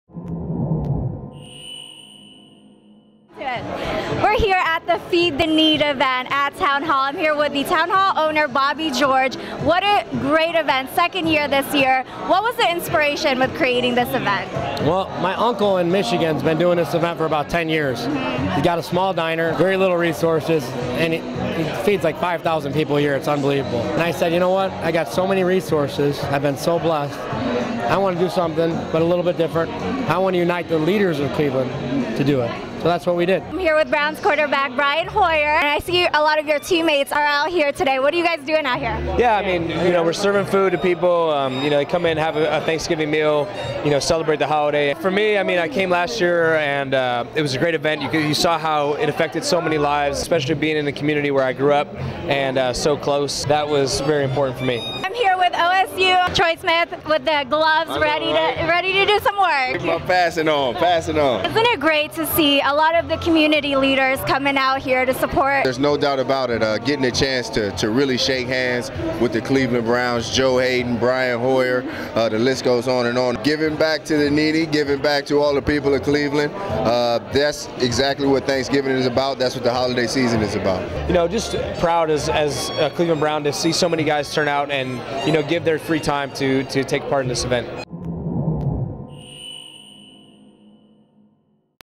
Cleveland Browns players lend a hand at 2nd annual "Feed the Need" event
The second annual &quot;Feed the Need&quot; event took place at Townhall, 1909 West 25th St., Tuesday where Cleveland celebrities helped pass out food to the needy. We talked with Browns QB Brian Hoyer, former OSU player Troy Smith and Townhall owne